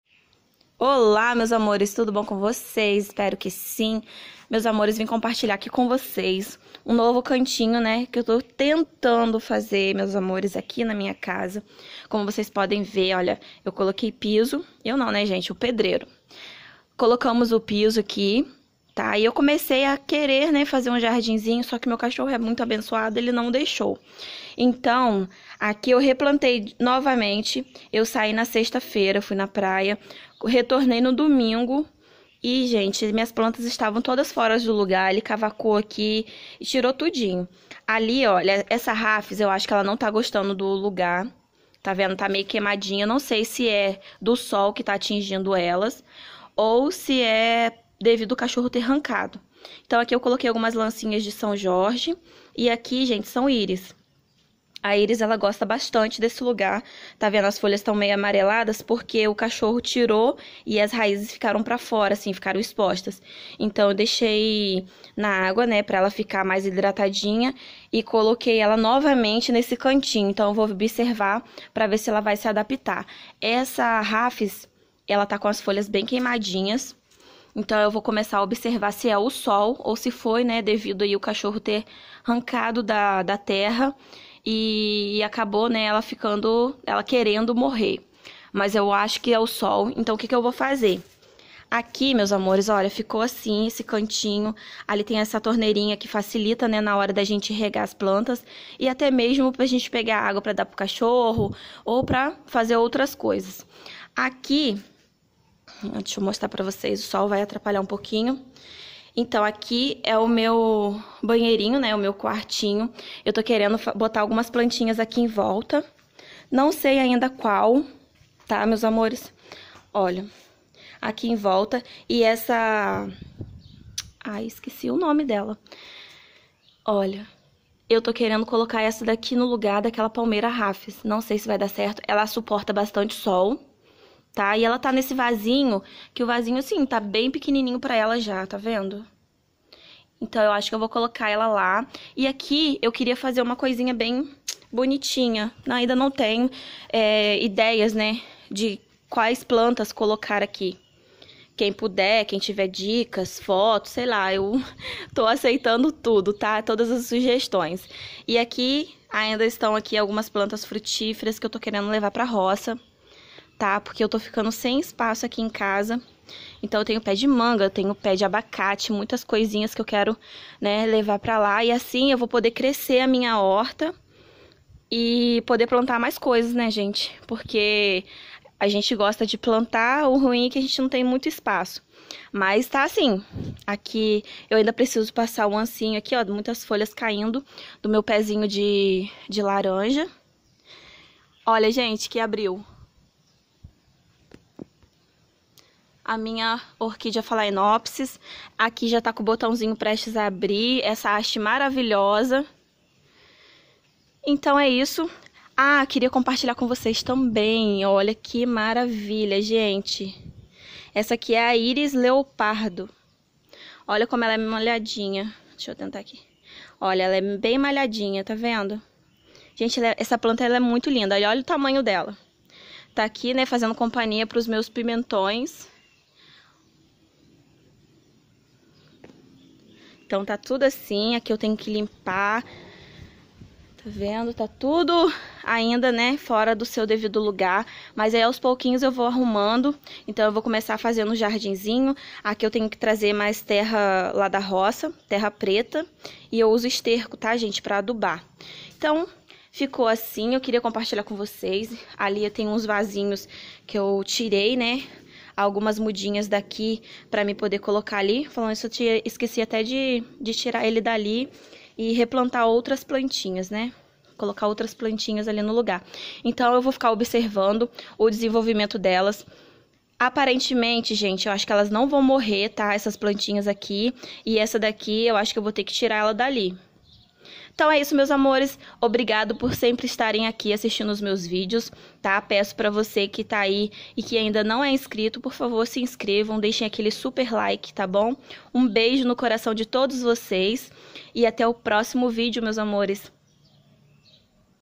Tentei fazer um jardim
Um lindo dia para todos, tentei fazer um jardim porém meu cachorro muito levado arrancou todas as plantinhas do lugar.
E ficou assim🤦🏽‍♀️.